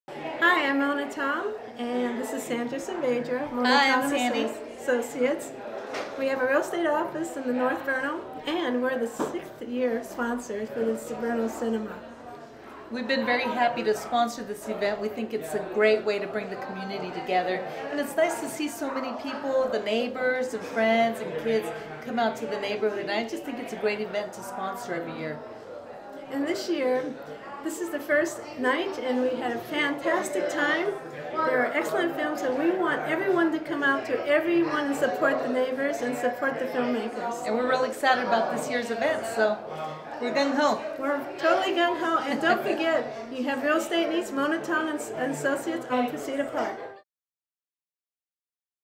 Sponsors of the event BHOC
Interview with sponsors  of the BHOC for years Mona Tong and Sandra Saavdra.